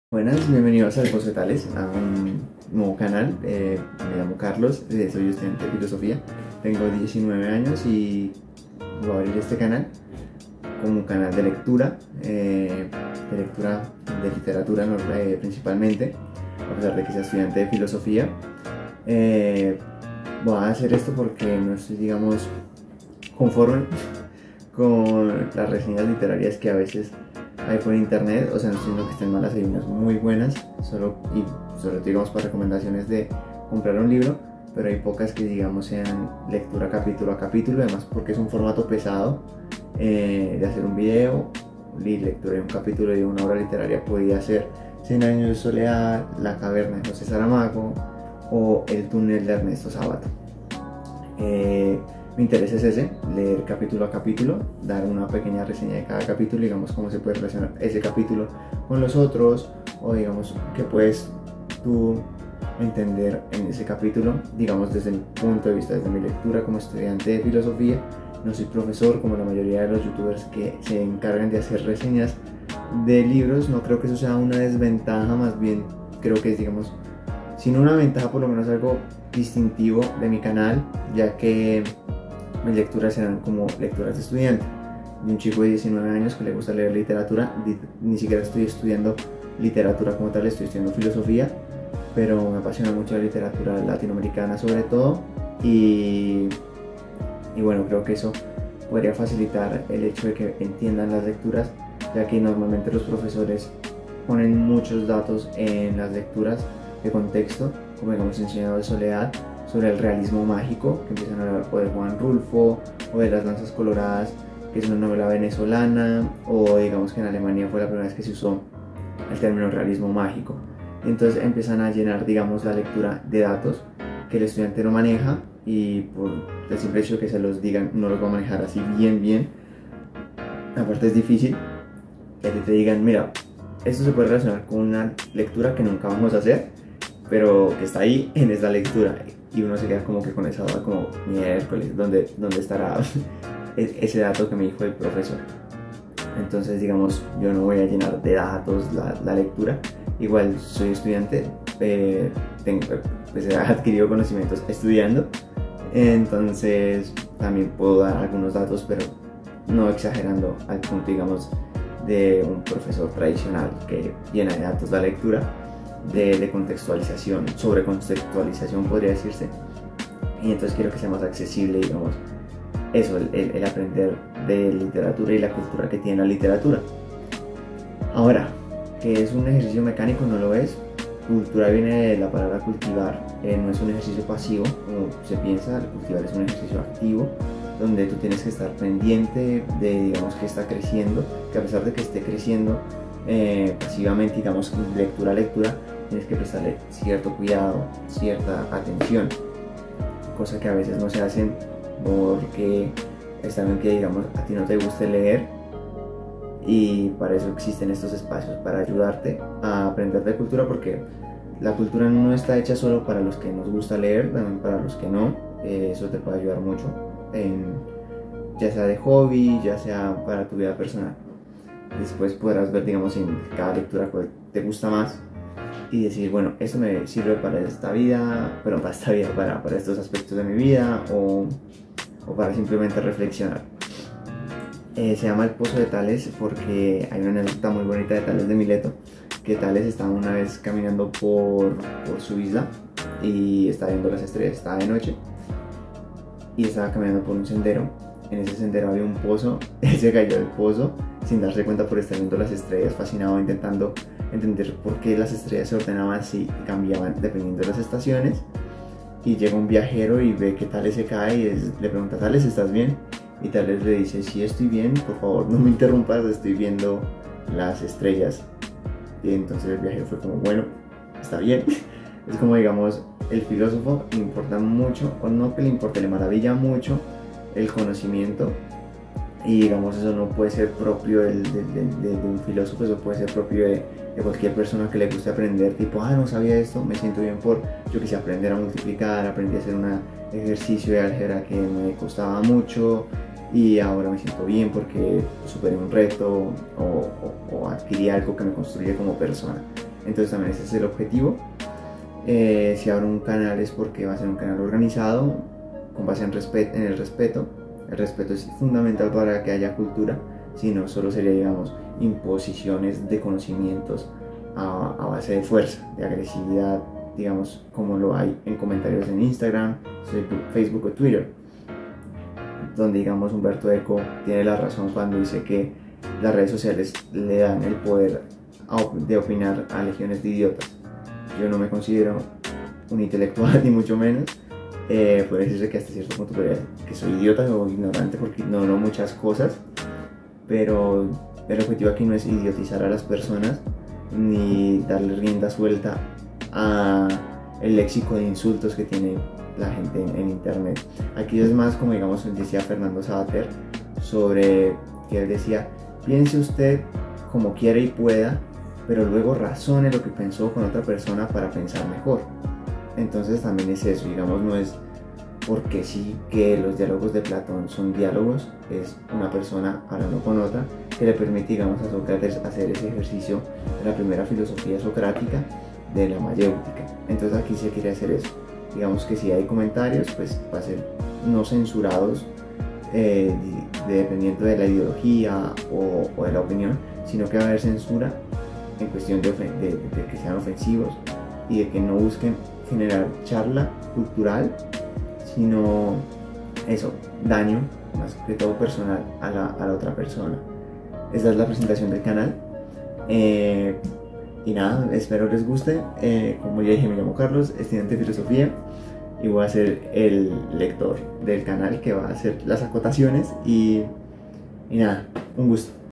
Bienvenidos al Pozo de Tales
Muy buenas, ¿cómo están? Espero que muy bien.
Traigo el primer video del canal, con gran emoción diré. Aquí presento la meta y la idea del formato a manejar en este canal de lectura. Subo ciertas obras de arte. Dos de ellas del movimiento del realismo en Europa (Las espigadoras y Buenos días, señor Coulbert). Intento que esas imágenes hagan el video más dinámico, o pedagógico. Que se puedan relacionar con el mensaje que se quiere dar. Espero les guste el formato.

Un saludo.

Por cierto, cuando salen Umberto Eco o Fernando Savater, abajo dice "Sacado de". Ahí se pone la bibliografía de la cita que se dice en el video.